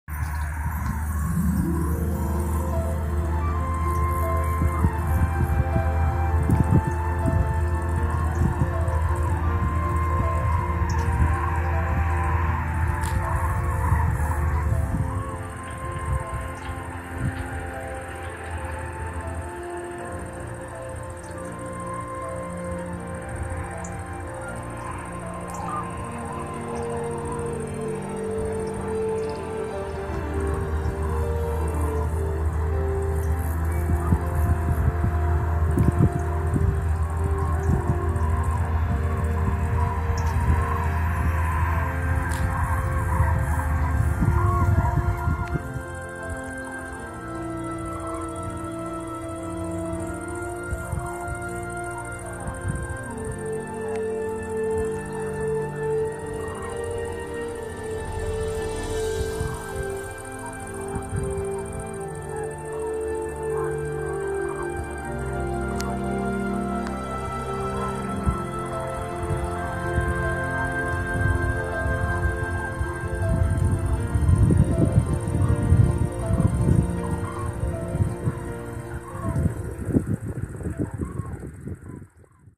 Indian Summer
Just a test video! Still, here are a few beautiful moments on a glorious autumn day at the end of October.